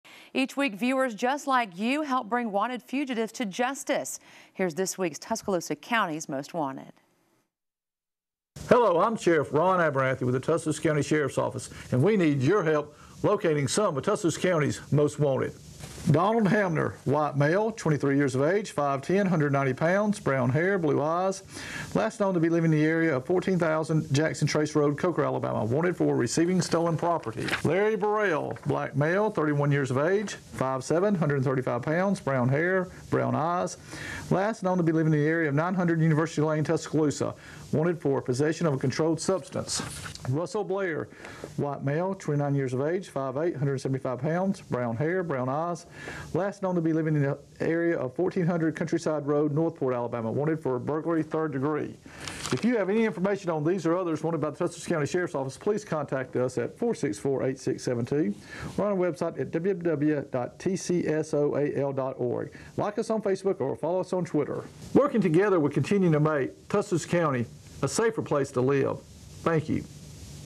Tuscaloosa's Most Wanted: March 2, 2016
TUSCALOOSA – Each week, WVUA 23 viewers help bring wanted fugitives to justice.

This week, the Tuscaloosa County Sheriff’s Department needs help finding:

Donald Brandon Hamner, 23, white male, 5 feet, 10 inches tall, 190 pounds, brown hair and blue eyes. His last known address is in the 14000 Jackson Trace Road in Coker. Wanted  for receiving stolen property 2nd degree.
Larry Deon Burrell, 31, black male, 5 feet, 7 inches tall, 135 pounds with black hair and brown eyes. His last known address is in the 900 University Lane in Tuscaloosa. He's wanted for possession of a controlled substance.
Russell Gordon Blair, 29, white male, 5 feet, 8 inches tall, 175 pounds, brown hair and brown eyes. His last known address is in the 1400 of countryside Road in Northport. He's wanted for Burglary 3rd degree, theft of property 2nd degree, and possession of a controlled substance.
If you have any information on these or others wanted by the Tuscaloosa County Sheriff’s Office, please call or visit tscoal.org. You can also visit the Sherrif’s Office on Facebook or Twitter.